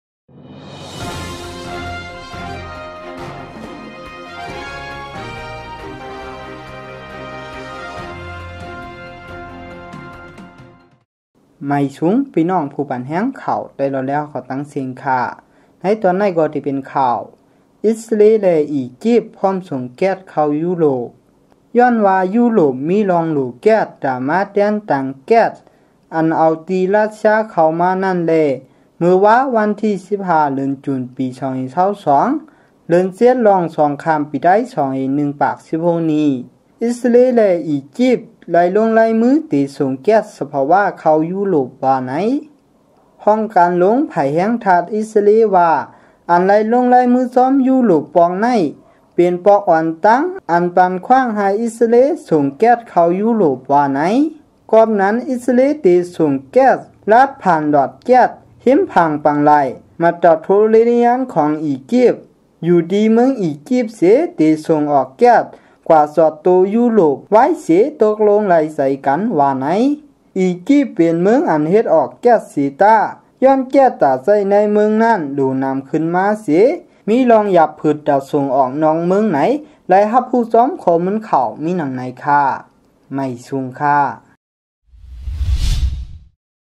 ၶၢဝ်ႇဝၼ်းထီႉ − 16.06.2022
ဢိတ်ႉသရေးလႄႈ ဢီႇၵျိပ်ႉ ၽွမ်ႉသူင်ႇၵႅတ်ႉ ၶဝ်ႈယူးရူပ်ႇ